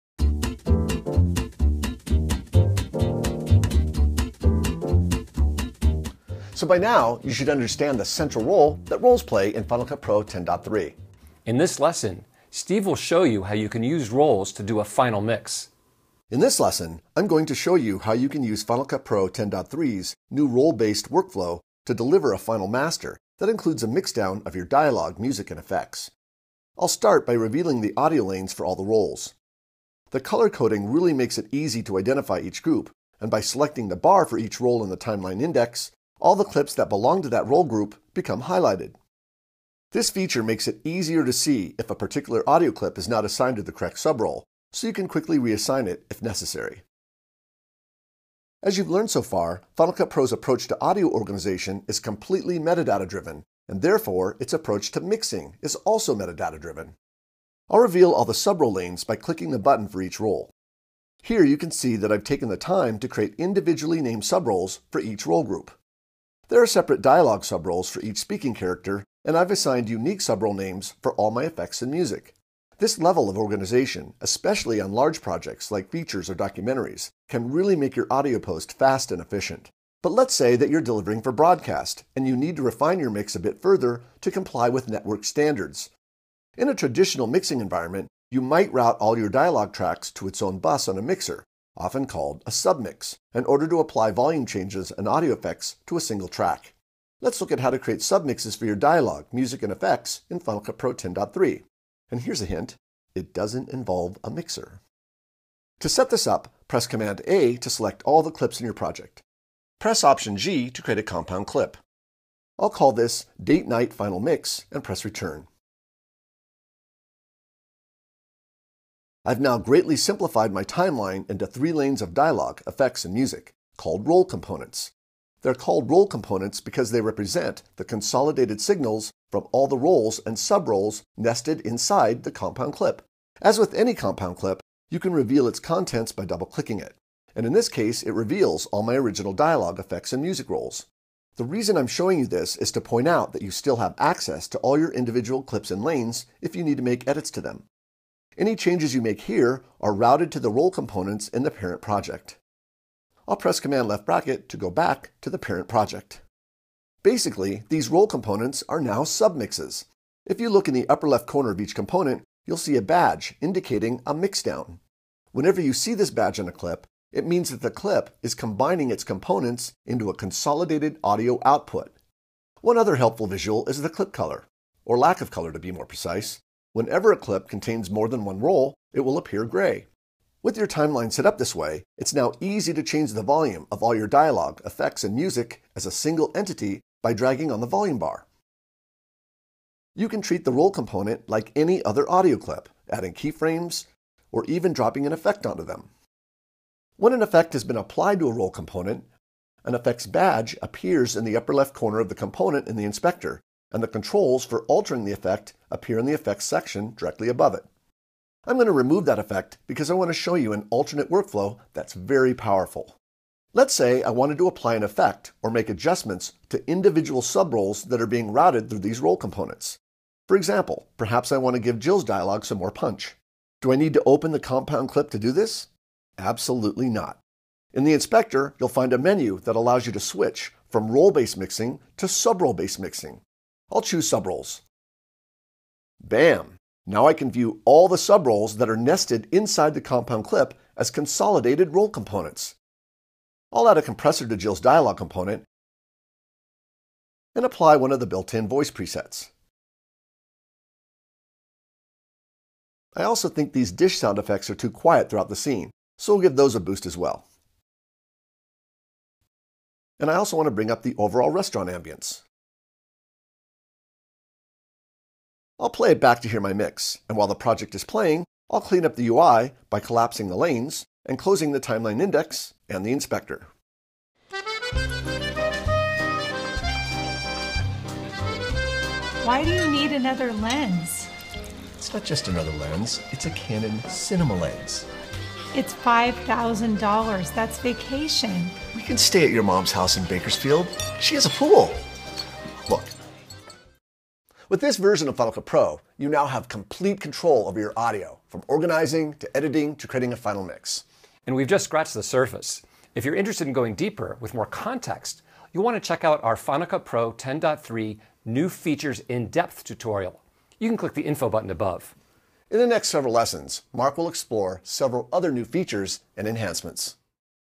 Final Cut Pro 10.3 New Features Lesson 4: Mixing with Roles
Final Cut Pro 10.3 is here and it’s full of new features, workflow improvements and editing enhancements. Learn how to use Roles to create sub-mixes of your dialogue, music and effects, then apply audio effects to Roles or Role Components for delivering final masters.

The BEST Final Cut Pro X Training on the planet is found here: